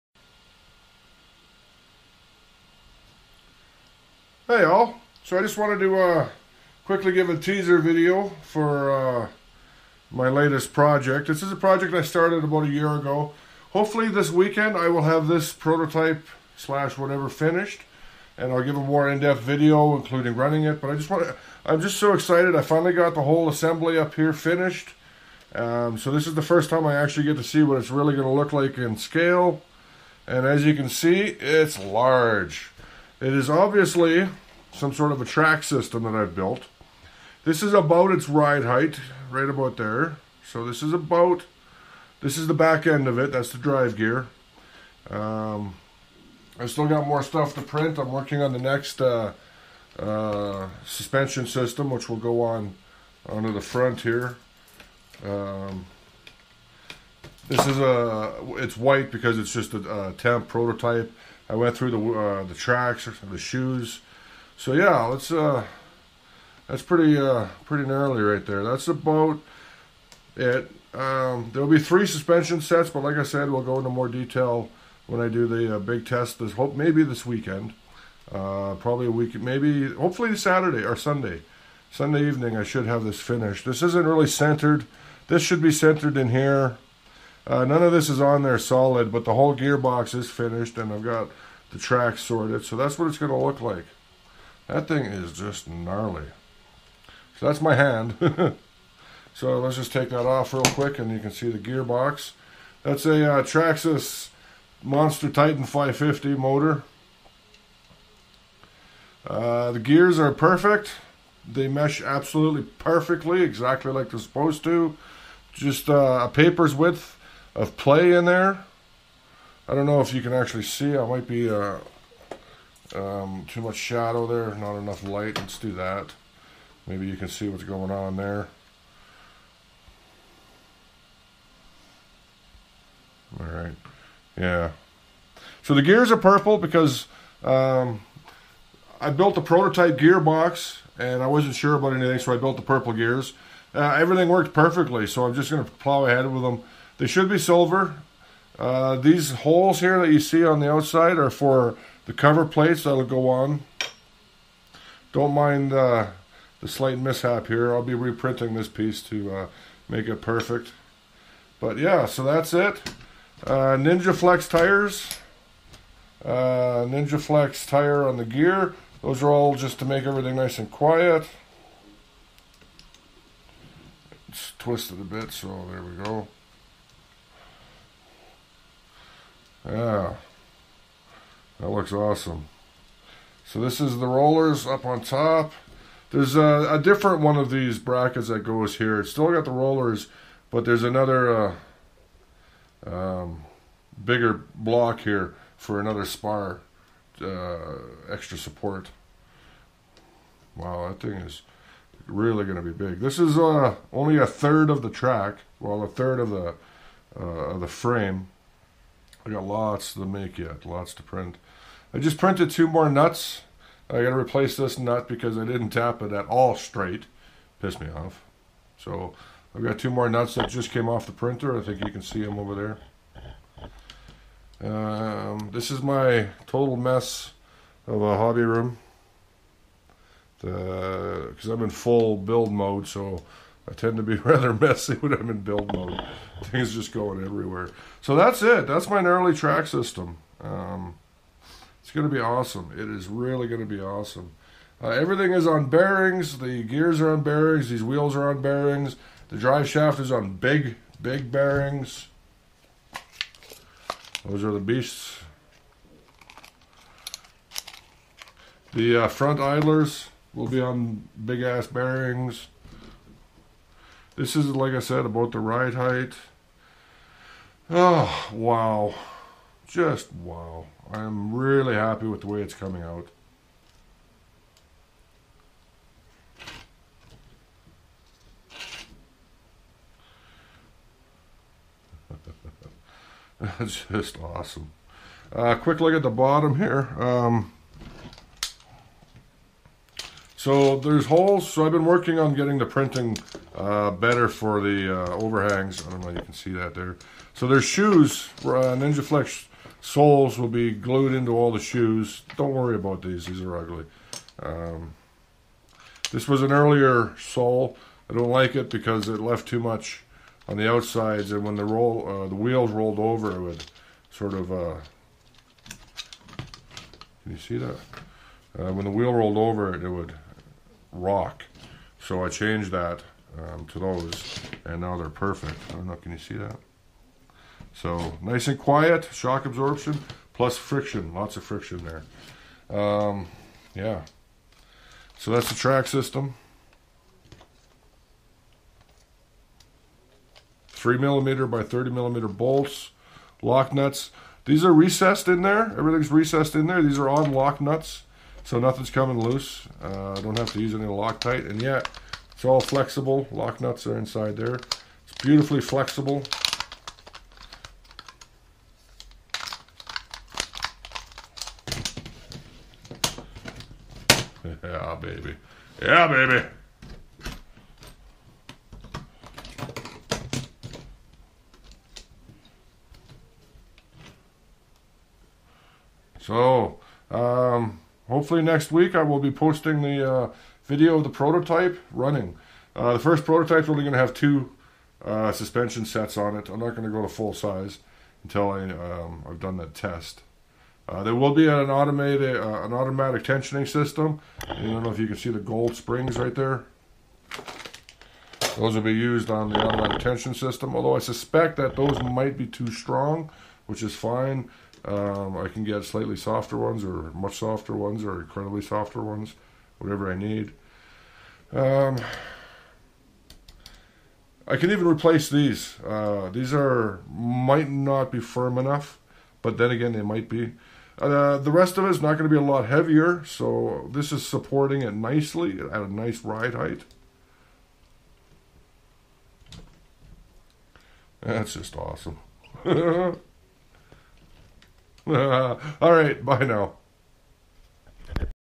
Early view of 3D Printed tank track system
Just a preview of the tank track system I'm developing.  It's almost entirely 3D printed.  The main backbone is a fiberglass tube.
Looking forward to completing the 2/3 prototype this weekend and performing a full powered test of the track.